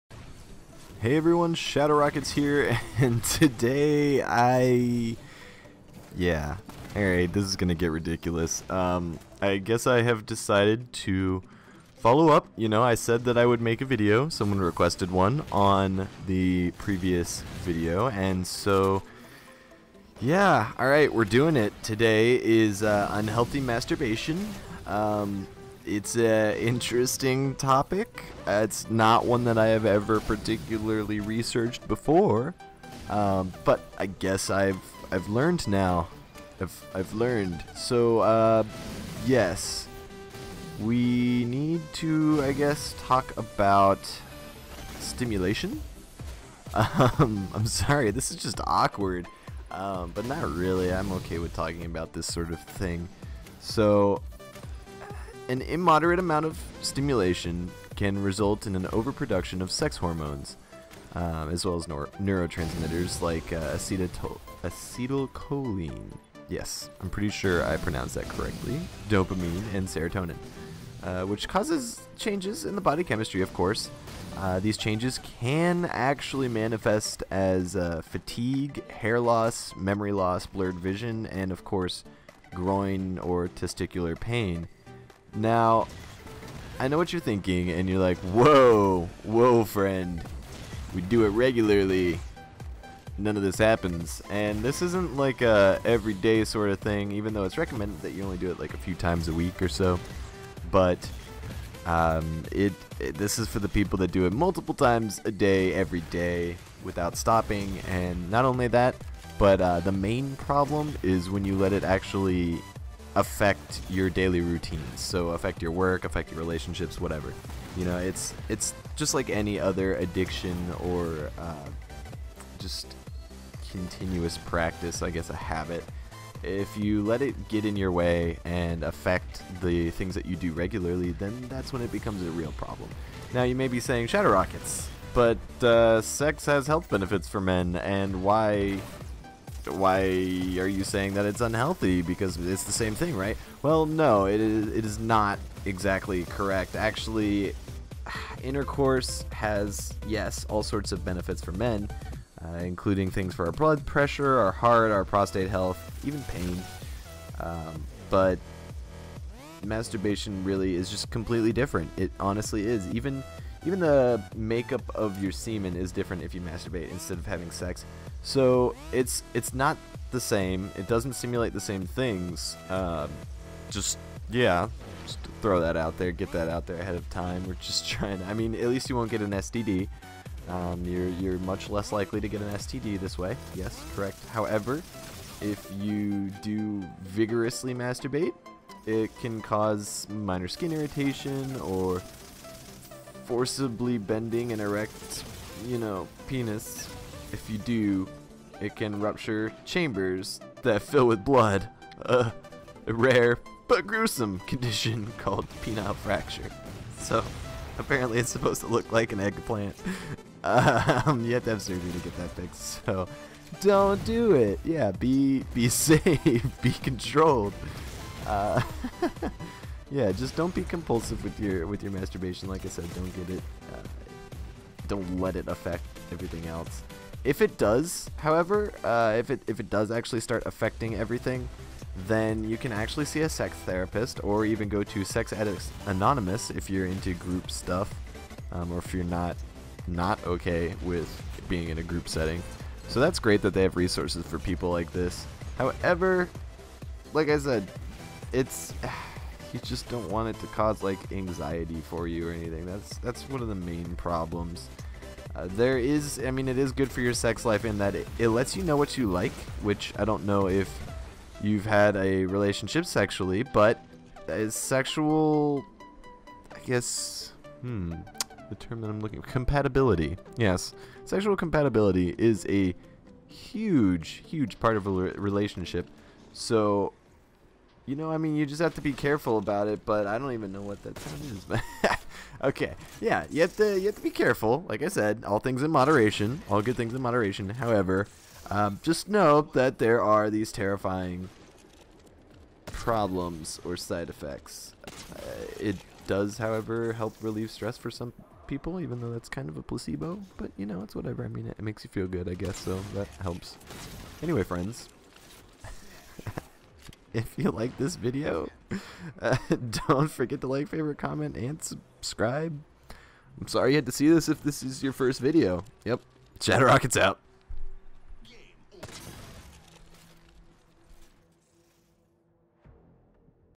The Unhealthy Side of Masterbation
How about those no scopes at the end?! WARNING: This is a mature commentary. While I can't believe I did this, I do acknowledge that this is an important topic and should be taken seriously! I tried out premiere pro today for the first time so tell me how you feel about the quality!